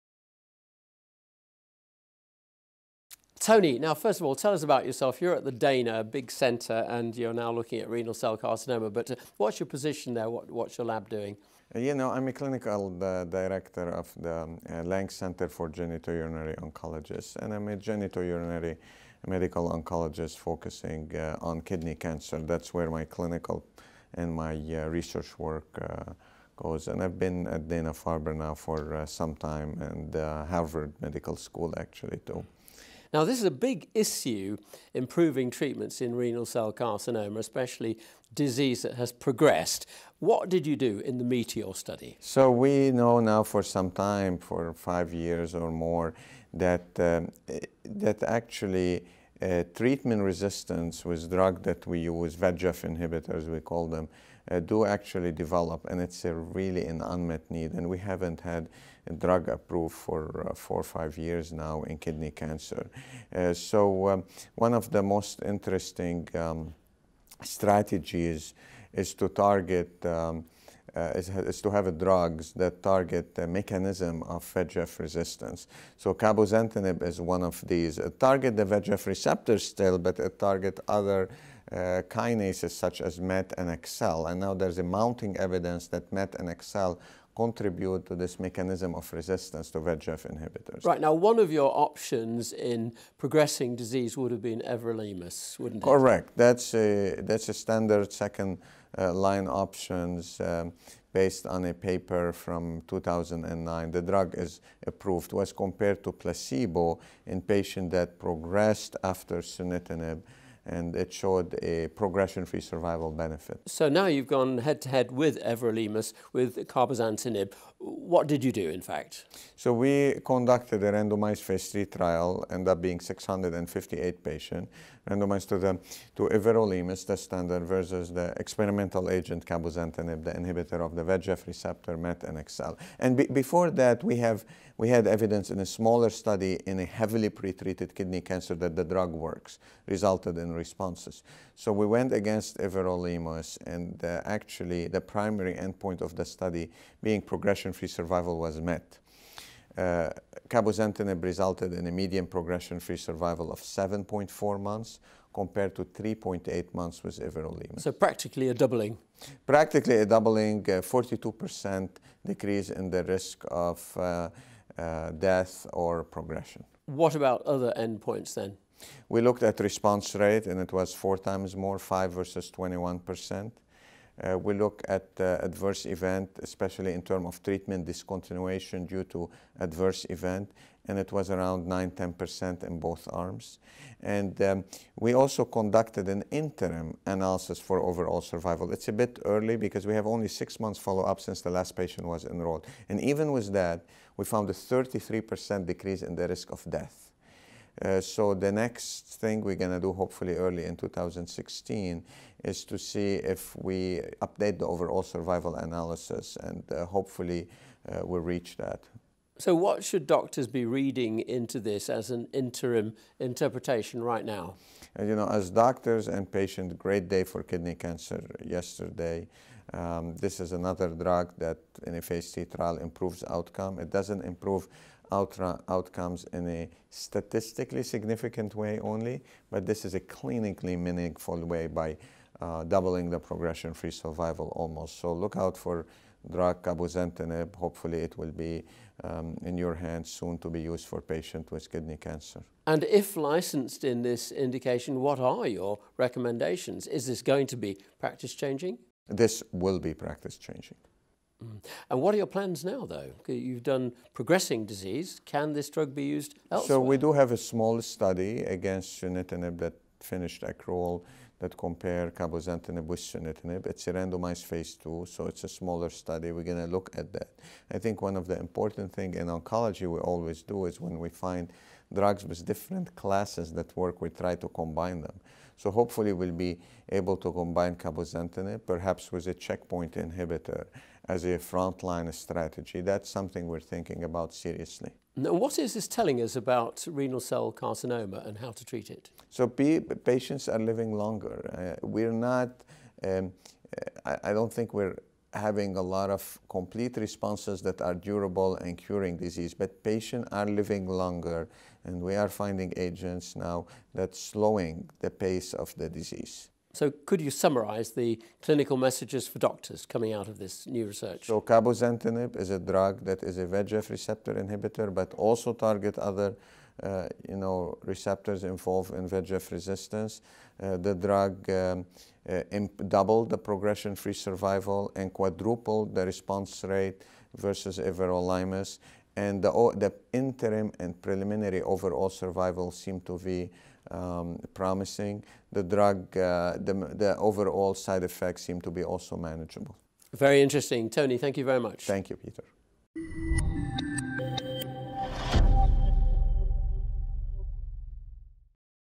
Cabozantinib improves advanced kidney cancer survival: METEOR trial results
Prof Choueiri talks to ecancertv at ECC 2015 about how patients with advanced kidney cancer live for nearly twice as long without their disease progressing if they are treated with cabozantinib, a small molecule tyrosine kinase inhibitor (TKI) that targets c-MET, VEGFR2 and AXL..

In the interview he discussed the results of the open-label METEOR trial, which compared the TKI against everolimus, a standard of care in mRCC.